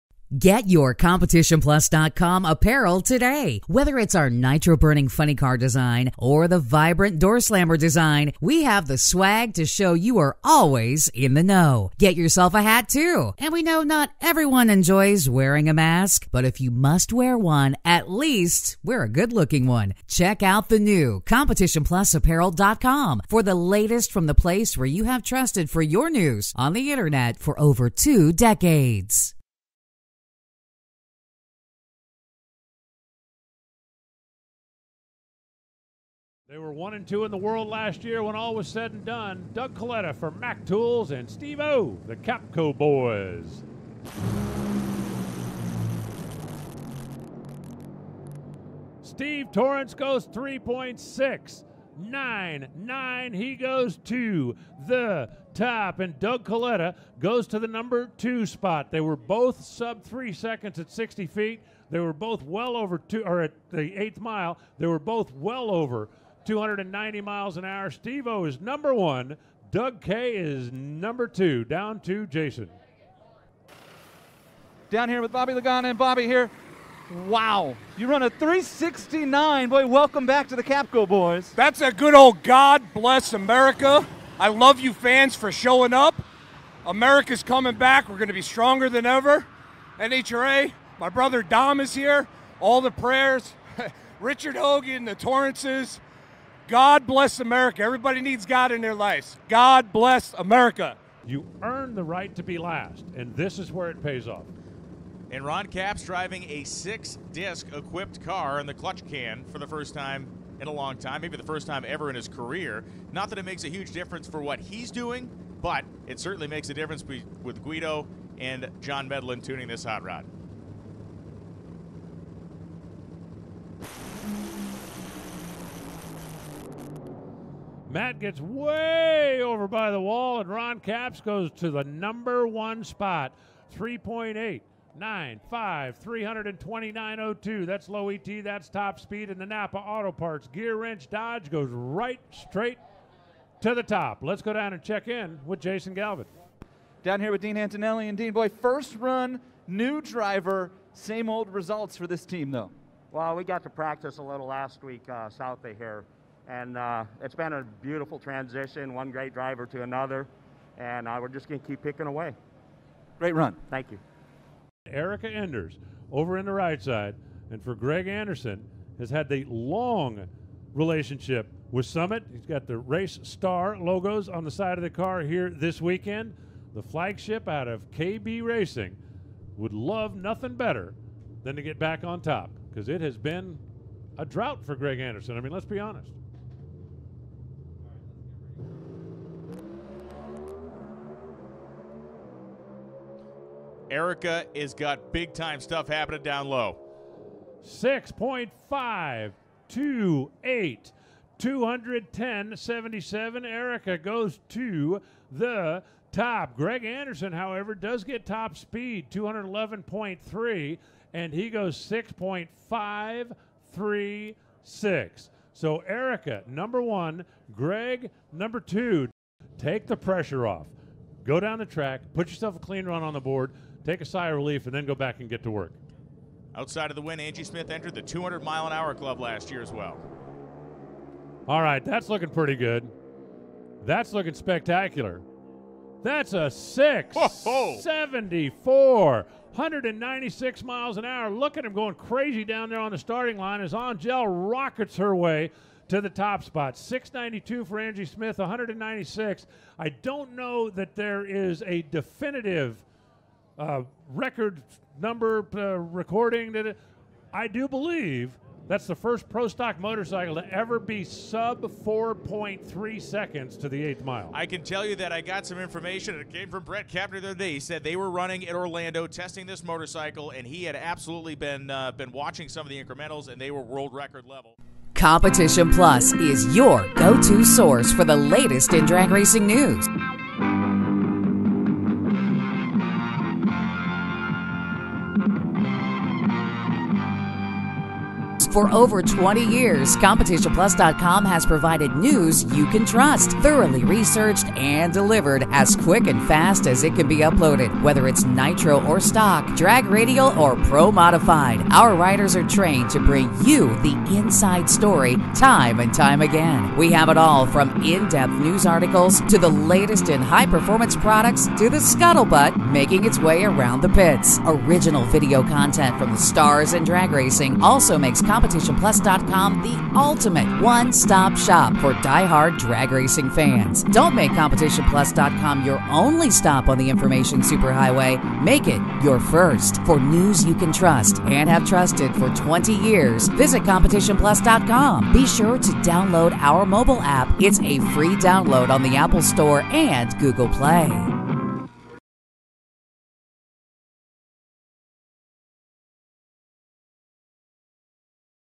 '21 NHRA GATORNATIONALS - LEADERS ON FIRST DAY OF NEW SEASON
Even with two new crew chiefs, Ron Capps started his 2021 NHRA Camping World Drag Racing Series season on a high note on Friday, powering to the provisional No. 1 spot in qualifying in Funny Car at the season-opening 52nd annual AMALIE Motor Oil NHRA Gatornationals at Gainesville Raceway.

Steve Torrence (Top Fuel), Erica Enders (Pro Stock) and Angelle Sampey (Pro Stock Motorcycle) are also provisional No. 1 qualifiers in their respective categories at the first race of the 2021 NHRA Camping World Drag Racing Series season.

Our hard working team at CompetitionPlusTV have compiled the top runners in each of the nitro divisions from testing at the PRO Winter Warm-up at Palm Beach International Raceway.